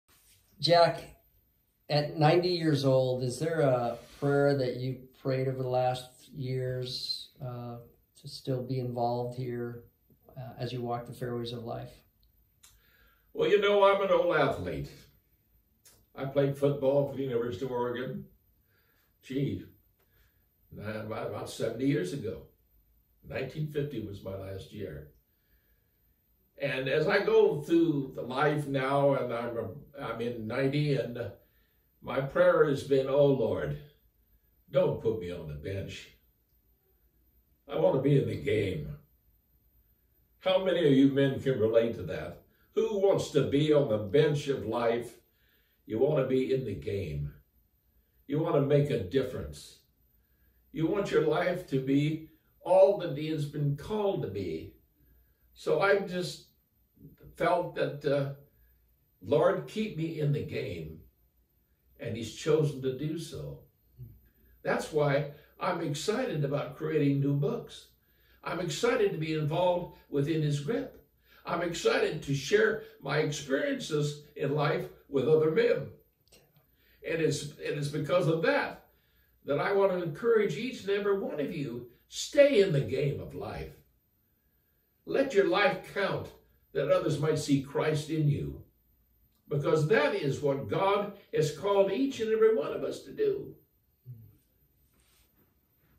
My 92 year old mentor, Jack's powerful daily prayer to God about keeping him in the game of life.
Be inspired with this message from Mr Countryman.  Jack still has a lot of game left in him.